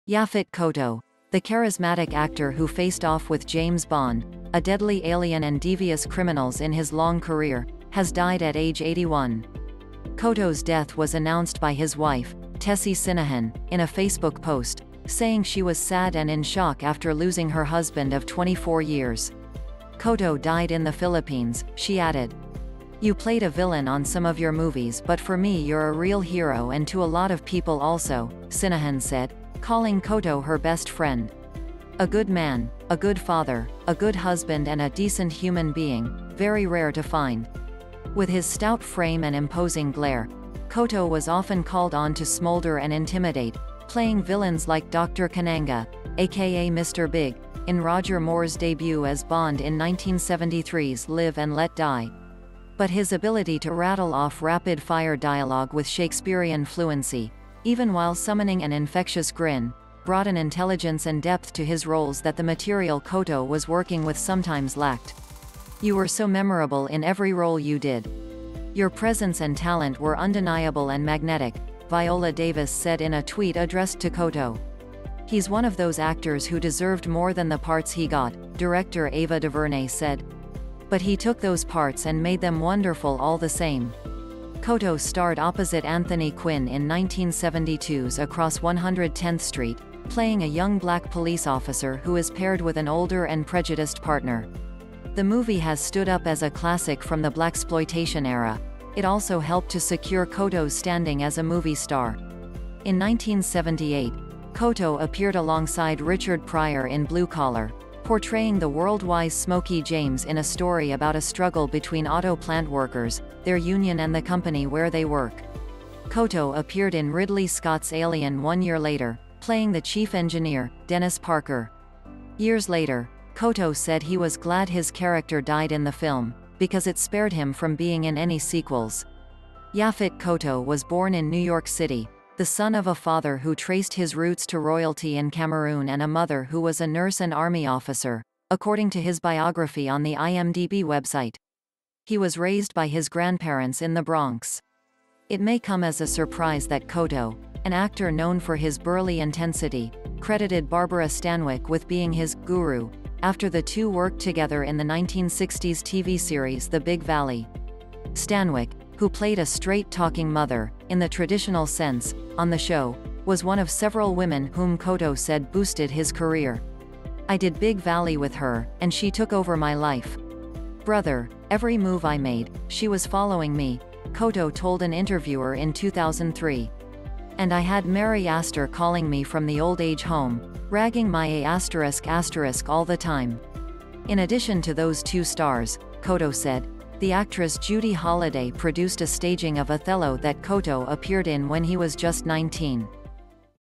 Yaphet Kotto Magnetic Actor With A Long And Varied Career Dies At 81
Yaphet Kotto, the charismatic actor who faced off with James Bond, a deadly alien and devious criminals in his long career, has died at age 81. Kotto's death was announced by his wife, Tessie Sinahon, in a Facebook post, saying she was sad and in shock after losing her husband of 24 years. Kotto died in the Philippines, she added."You played a villain on some of your movies but for me you're a real hero and to a lot of people also," Sinahon said, calling Kotto her best friend. "A good man, a good father, a good husband and a decent human being, very rare to find."With his stout frame and imposing glare, Kotto was often called on to smolder and intimidate, playing villains like Dr. Kananga, a.k.a. Mr. Big, in Roger Moore's debut as Bond in 1973's Live and Let Die. But his ability to rattle off rapid-fire dialogue with Shakespearean fluency, even while summoning an infectious grin, brought an intelligence and depth to his roles that the material Kotto was working with sometimes lacked."You were so memorable in every role you did. Your presence and talent were undeniable and magnetic," Viola Davis said in a tweet addressed to Kotto."He's one of those actors who deserved more than the parts he got," director Ava DuVernay said. "But he took those parts and made them wonderful all the same."Kotto starred opposite Anthony Quinn in 1972's Across 110th Street, playing a young Black police officer who is paired with an older and prejudiced partner. The movie has stood up as a classic from the Blaxploitation era; it also helped to secure Kotto's standing as a movie star. In 1978, Kotto appeared alongside Richard Pryor in Blue Collar, portraying the world-wise Smokey James in a story about a struggle between auto plant workers, their union and the company where they work. Kotto appeared in Ridley Scott's Alien one year later, playing the chief engineer, Dennis Parker. Years later, Kotto said he was glad his character died in the film, because it spared him from being in any sequels. Yaphet Kotto was born in New York City, the son of a father who traced his roots to royalty in Cameroon and a mother who was a nurse and army officer, according to his biography on the IMDB website. He was raised by his grandparents in the Bronx. It may come as a surprise that Kotto, an actor known for his burly intensity, credited Barbara Stanwyck with being his "guru," after the two worked together in the 1960s TV series The Big Valley. Stanwyck, who played a straight-talking mother (in the traditional sense) on the show, was one of several women whom Kotto said boosted his career."I did Big Valley with her, and she took over my life. Brother, every move I made, she was following me," Kotto told an interviewer in 2003. "And I had Mary Astor calling me from the old-age home, ragging my a** all the time."In addition to those two stars, Kotto said, the actress Judy Holliday produced a staging of Othello that Kotto appeared in when he was just 19.